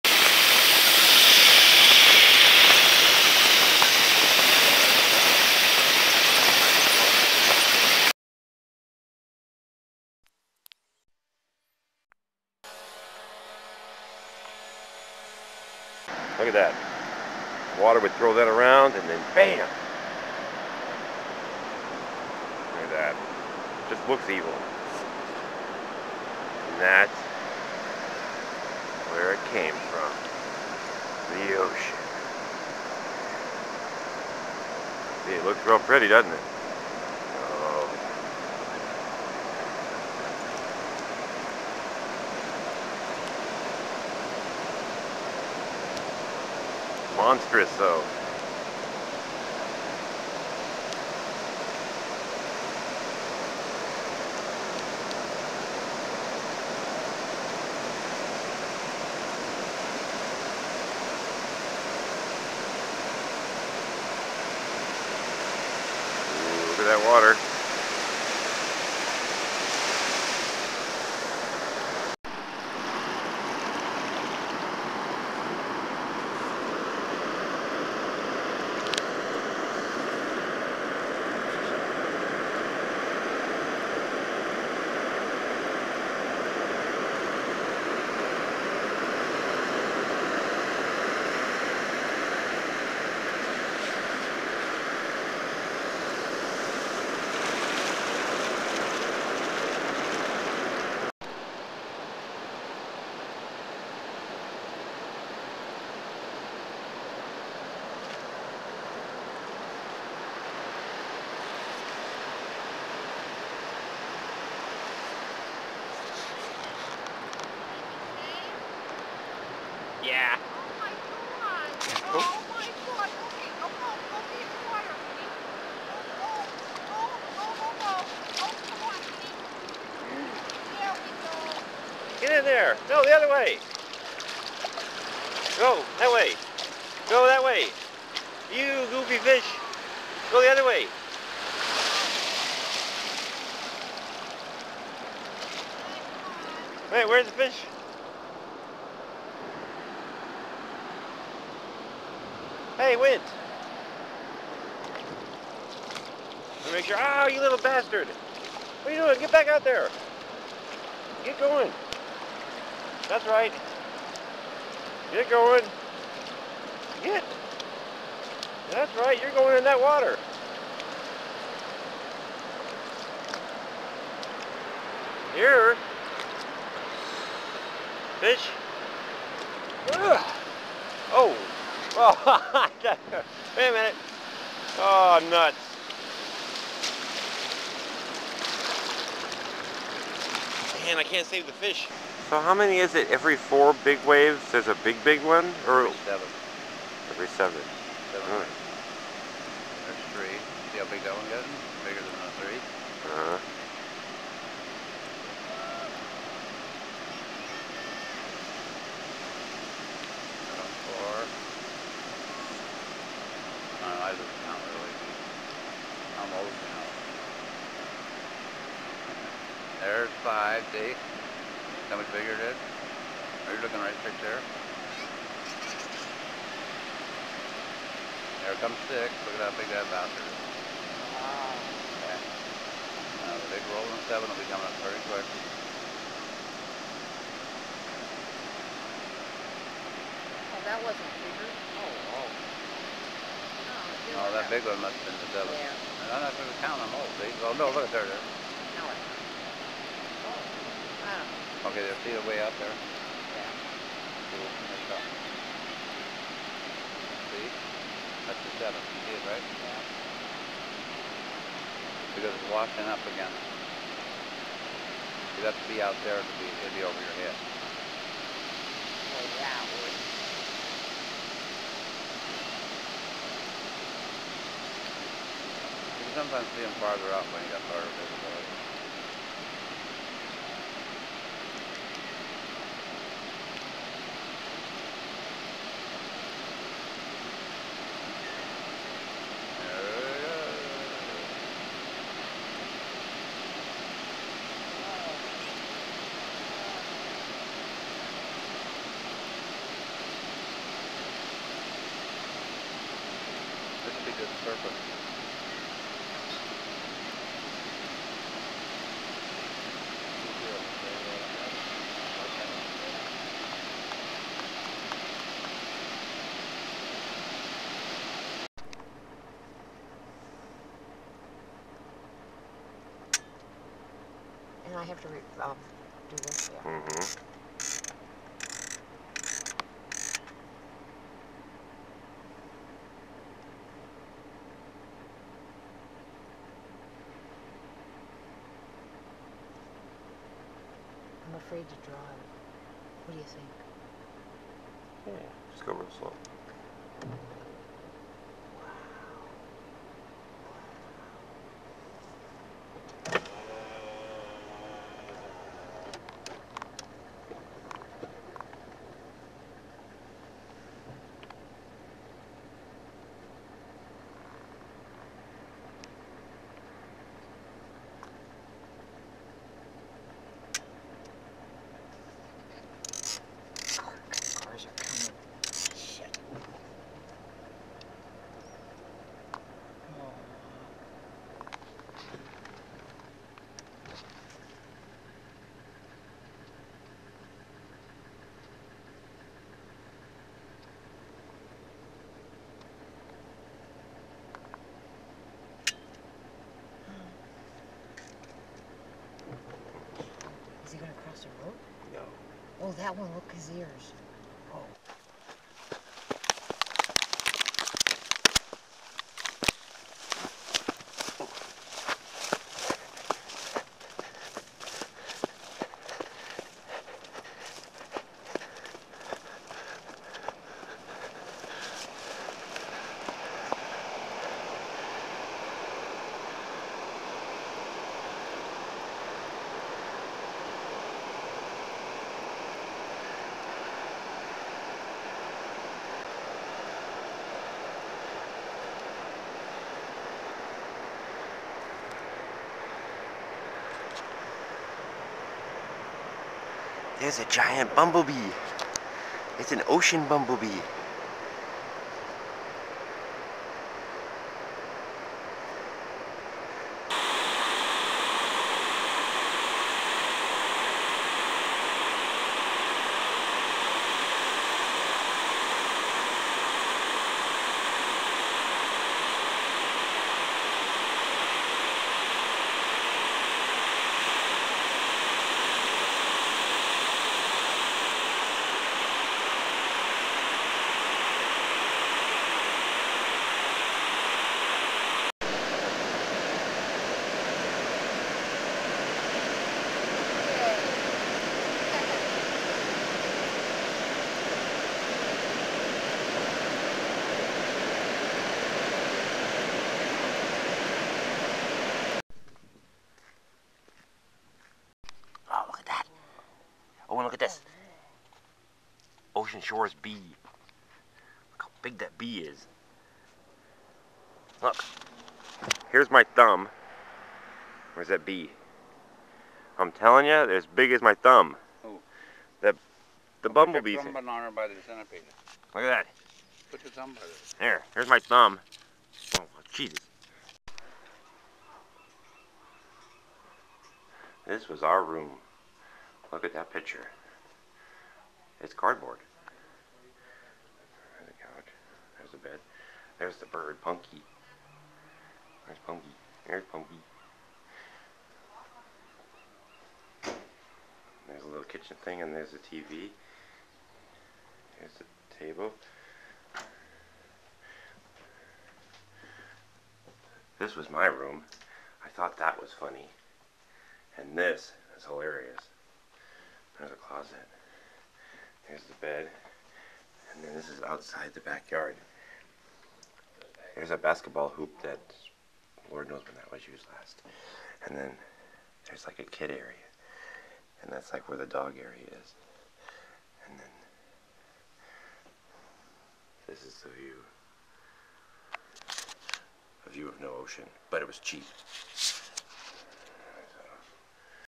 Ocean Shores 2010
Ocean Shores  is  130 miles from the house. I noticed familys on the beach having fun  I recorded them with a fear  that  giant waves or killer logs would cause harm.  One little black kid  had no one to play with so he tossed his shoe  in the water  and  it would  came back to him , he was playing catch with the ocean...  neat  but also a bit sad ,   A deer  had a little bird friend .
its july and hardley no one was at the Ocean it was odd,